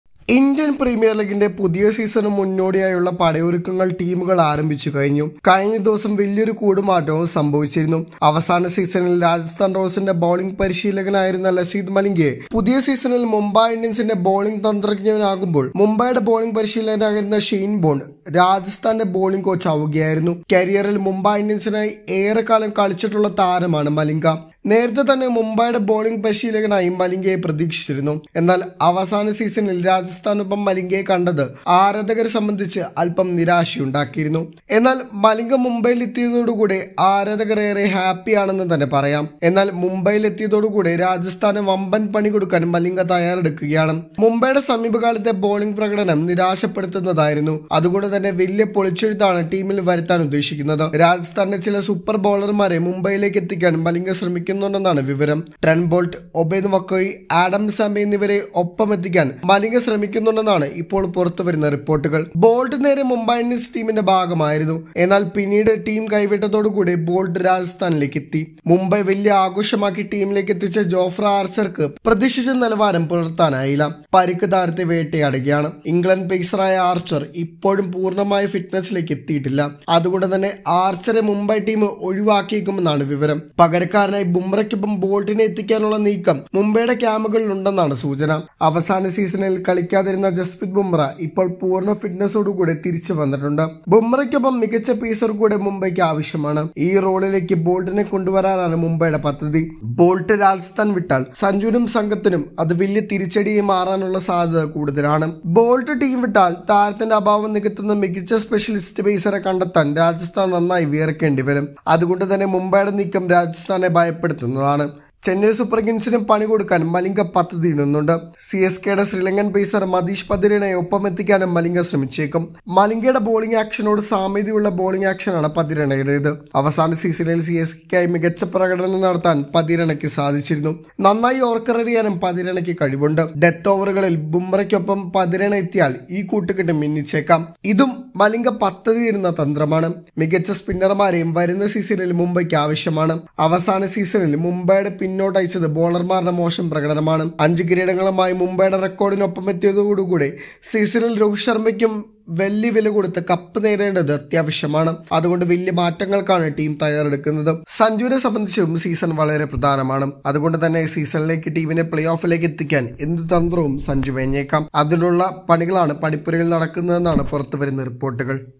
മലിംഗ ലക്ഷ്യമിടുന്ന വമ്പൻ അഴിച്ചുപണി Mumbai Indians To Bring Back Trent Boult
IPL 2023: Mumbai Indians plan with Lasith Malinga as the bowling coach | മുംബൈയിലെത്തിയതോടെ രാജസ്ഥാന് വമ്പന്‍ പണികൊടുക്കാന്‍ മലിംഗ തയ്യാറെടുക്കുകയാണ്. മുംബൈയുടെ സമീപകാലത്തെ ബൗളിങ് പ്രകടനം നിരാശപ്പെടുത്തുന്നതായിരുന്നു. അതുകൊണ്ടുതന്നെ വലിയ പൊളിച്ചെഴുത്താണ് ടീമില്‍ വരുത്താന്‍ ഉദ്ദേശിക്കുന്നത്